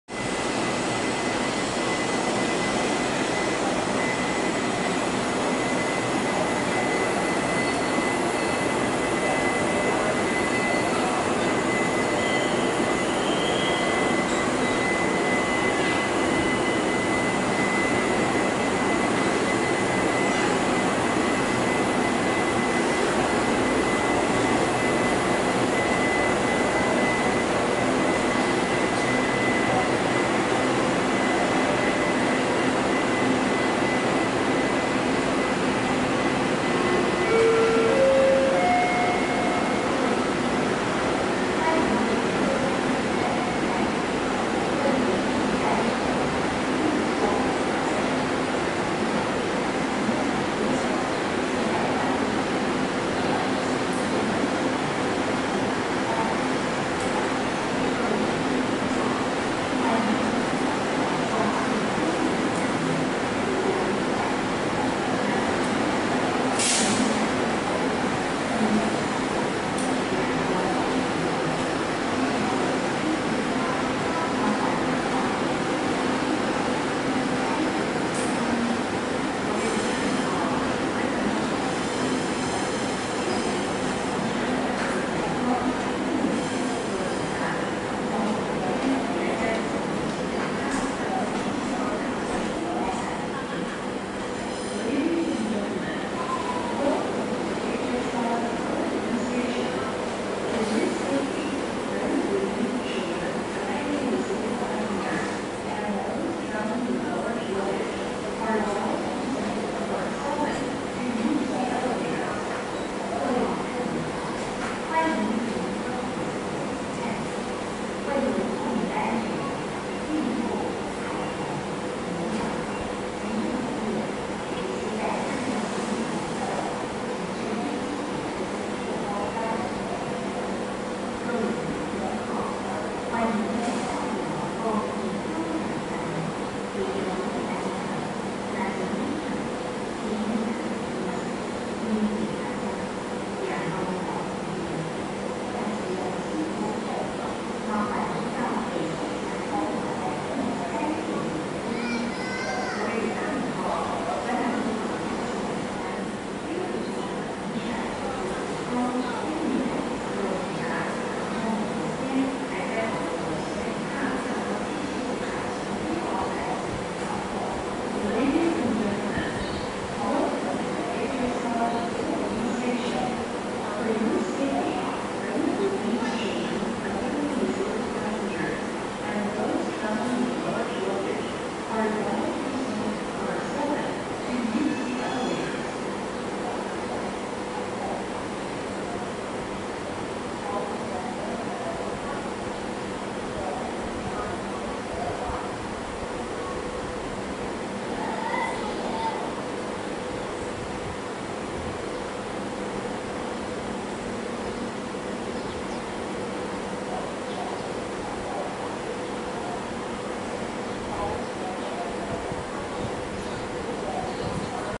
2019-08-10 新左營車站莒光號(本務E203)2A出站
莒光號  754車次  經由南迴線  臺東開往彰化
從3221次(前方停靠的EMU800)上下來就已經在出站了!!所以只能趕緊拍攝